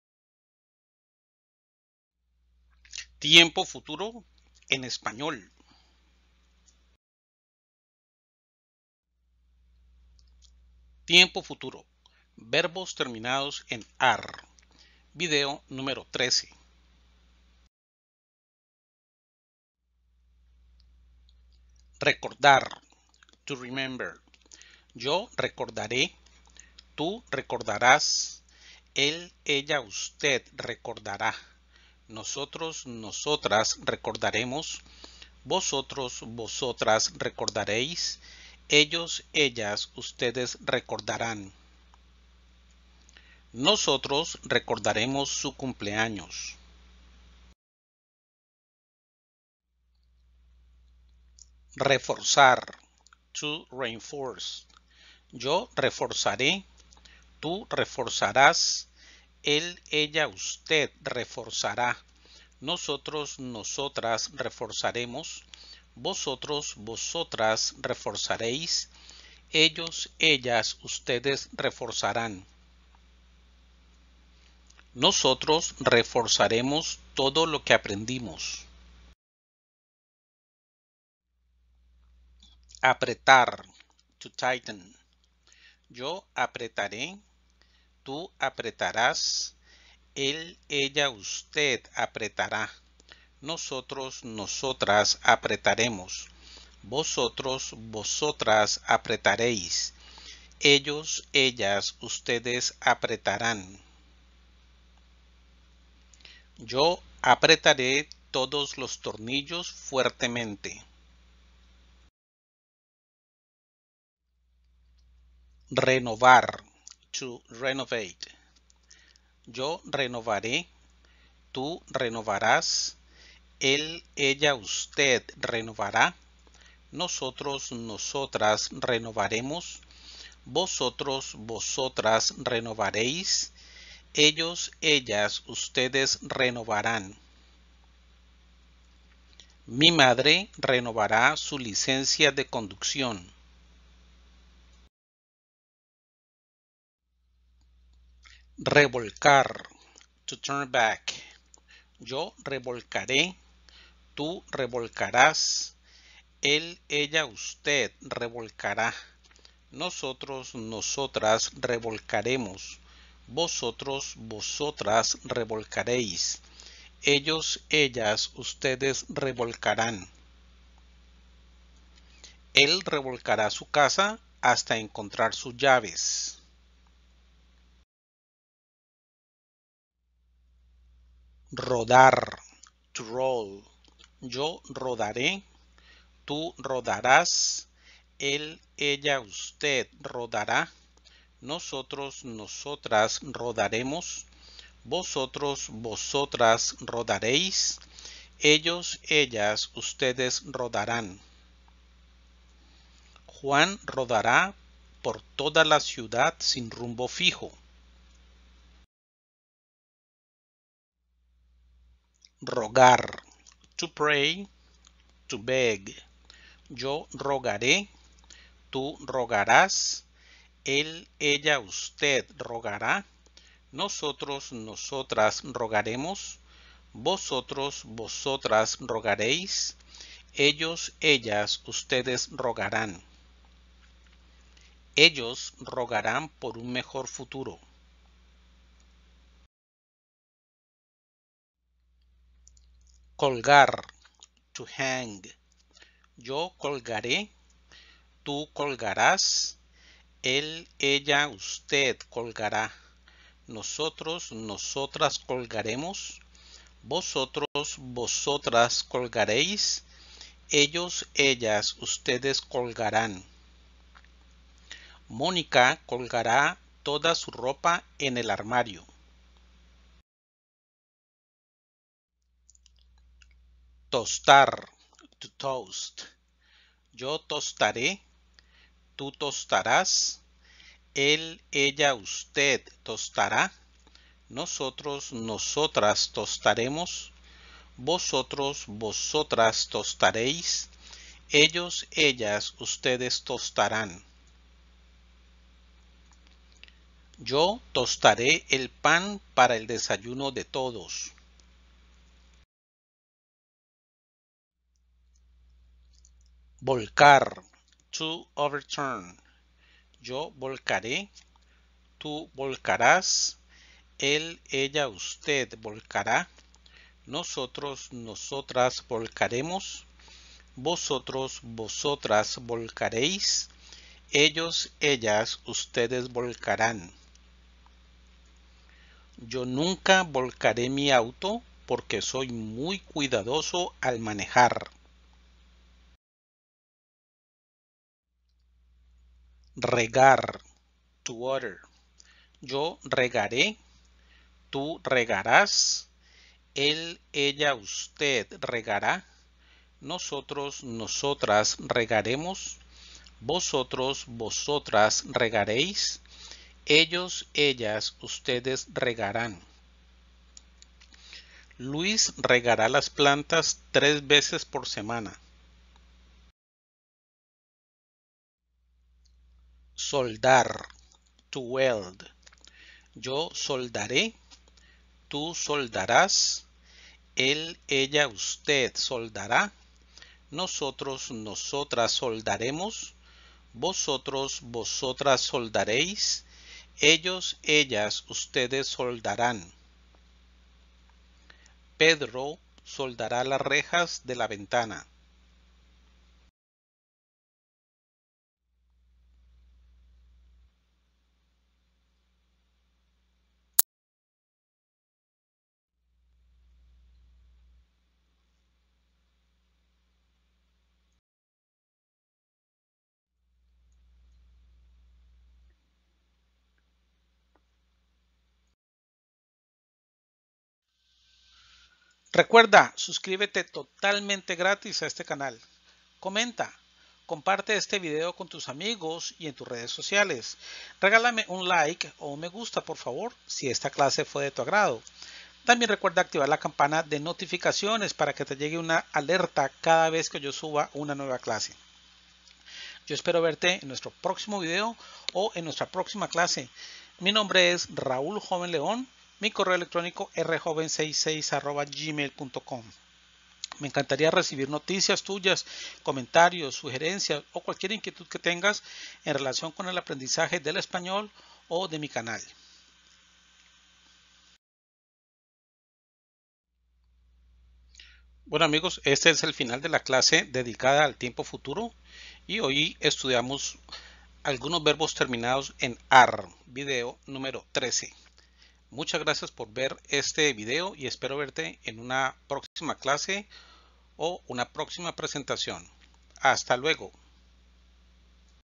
El tiempo futuro en español, 12 verbos regulares terminados en -AR (13)
Continuamos con el estudio del tiempo futuro en español. Hoy vamos a ver la formación estructural y algunas reglas para su uso correcto y su conjugación; también estudiaremos las terminaciones para cada uno de los pronombres personales. Espero que esta clase sea de tu gusto.
CANAL DE ESPAÑOL
 Magicalspanish (Youtube)
 @mymagicalspanish (Facebook)
 @JovenLen (Twitter)
 @ingleswithraul (Facebook)
 @inglés_joven (Twitter)